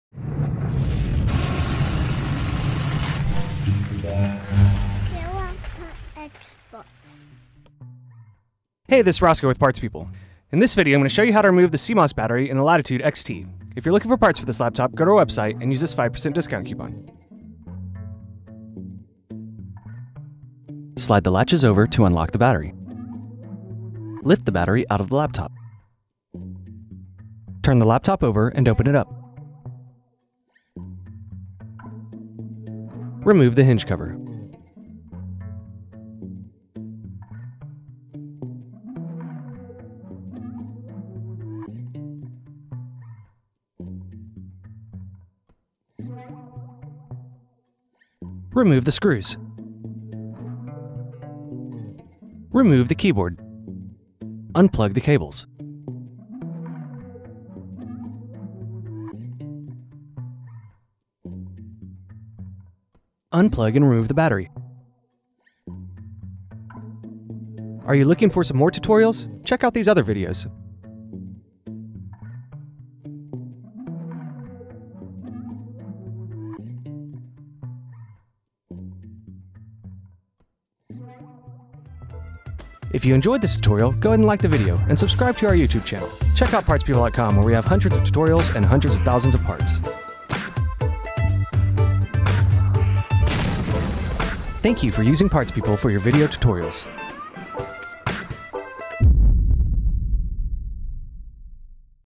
Dell Latitude XT CMOS Battery Video Tutorial Teardown
This Latitude XT video repair tutorial was brought to you